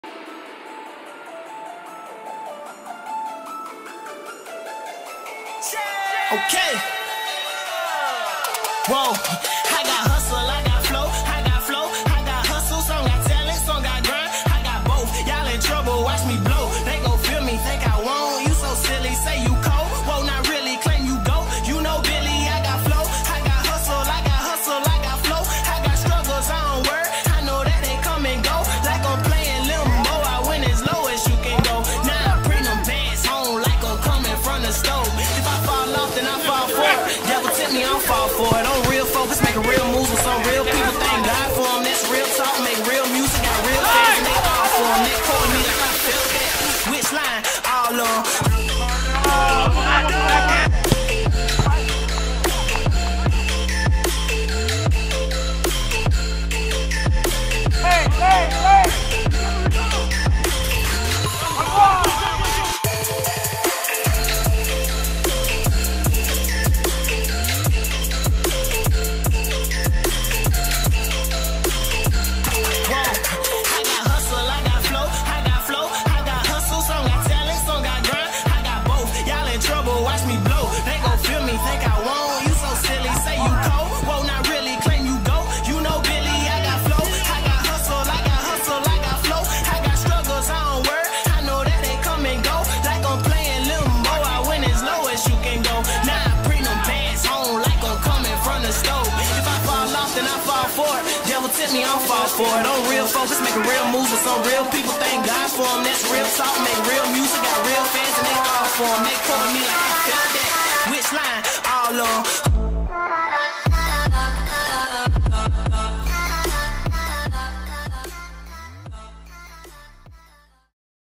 Episode 2: Chemistry in the making
The team is wrapping up their second week of preseason. Despite large amounts of heavy rain throughout the week, the squad continued to refine their skills and bring even greater attention to the tactical aspect of the game.